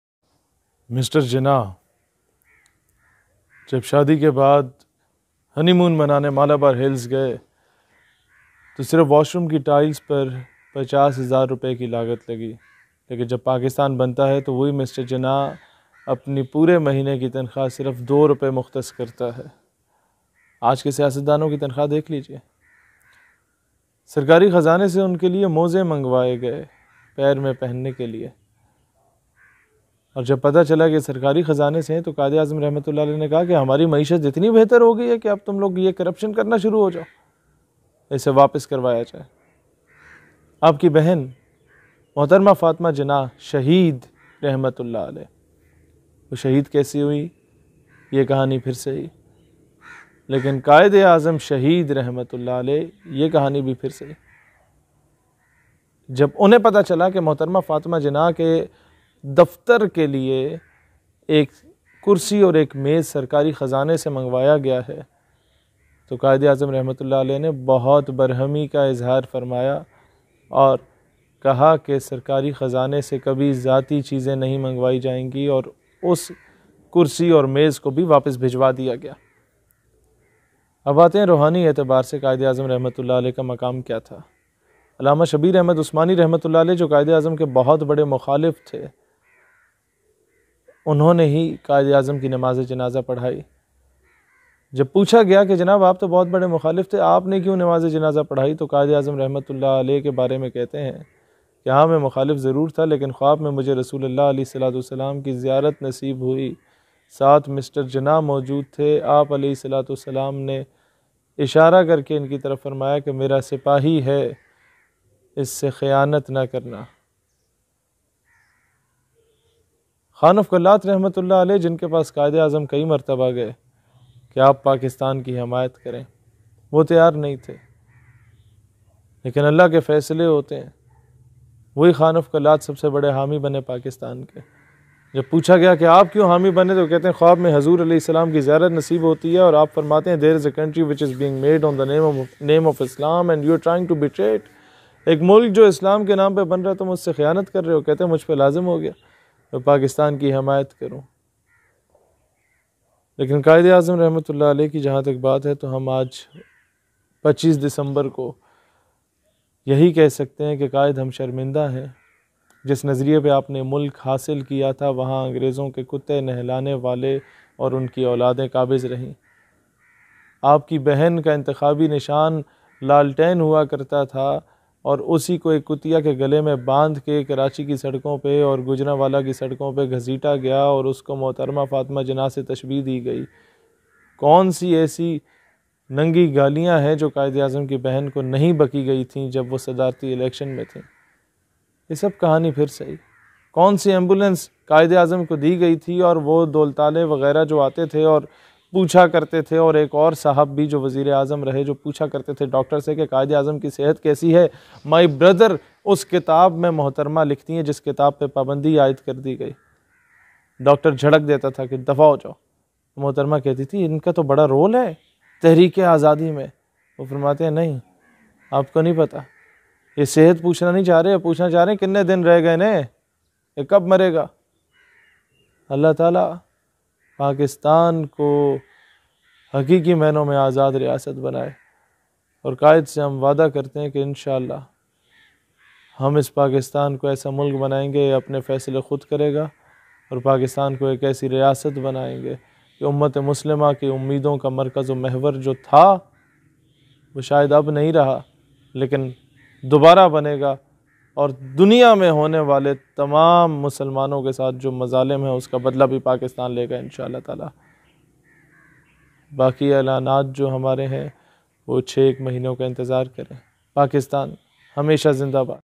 Quadeazam Muhammad ali jinnah day//quadeazam the great leader
Saheersialvi sahib the true patriotic man of pakistan.heis showing the real faces of the previous national heroes of pakistan and islam.please watch his videoos on my channel.thanks for watxhing